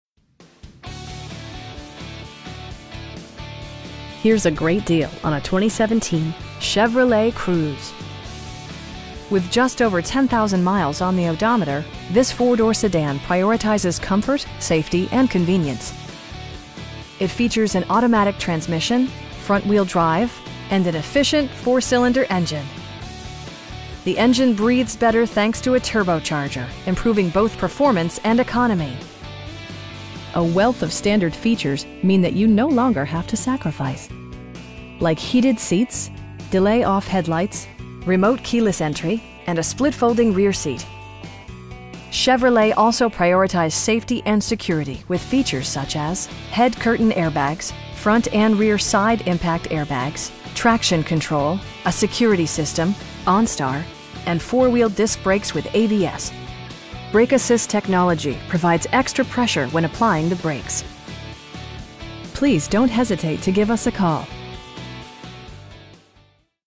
2017 Chevrolet Cruze LT in El Reno, OK 73036
Here's a great deal on a 2017 Chevrolet Cruze. With just over 10,000 miles on the odometer, this 4 door sedan prioritizes comfort, safety and convenience. It features an automatic transmission, front-wheel drive, and an efficient 4 cylinder engine. The engine breathes better thanks to a turbocharger, improving both performance and economy. A wealth of standard features means that you no longer have to sacrifice. Like heated seats, delay-off headlights, front bucket seats, air conditioning, telescoping steering wheel, remote keyless entry, cruise control, and a split folding rear seat. Chevrolet also prioritized safety and security with features such as: head curtain airbags, front and rear side impact airbags, traction control, a security system, onStar, and 4 wheel disc brakes with ABS. Brake assist technology provides extra pressure when applying the brakes. We know that you have high expectations, and we enjoy the challenge of meeting and exceeding them! Please don't hesitate to give us a call.